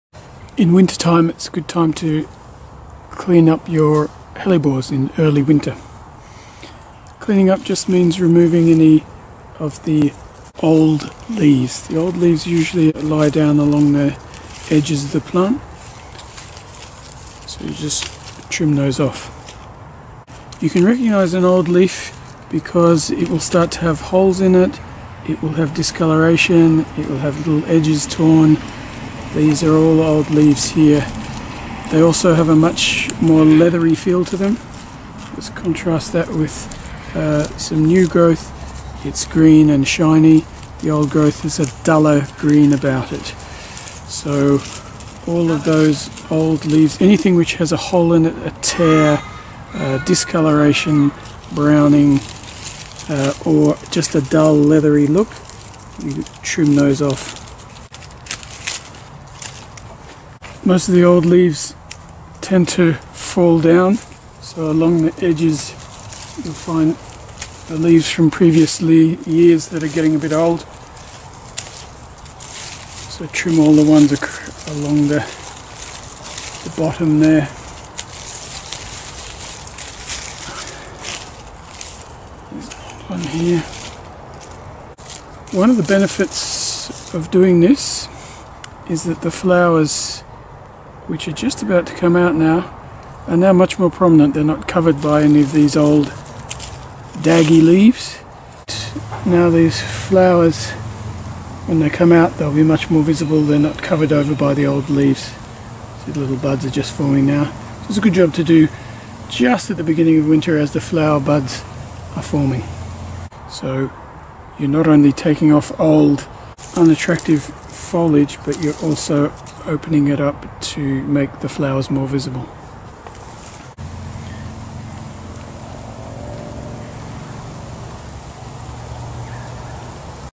Trimming Hellebores: show off your flowers and keep them tidy
Hellebores (Winter Rose) are a ray of sunshine in winter with their cheerful flowers.  To show off your hellebore flowers to their best potential, give the bush a trim in early winter. Watch this video to see how to make your Hellebores look smart.

Thanks to KENEPI for the beautiful photo of the helleborus niger.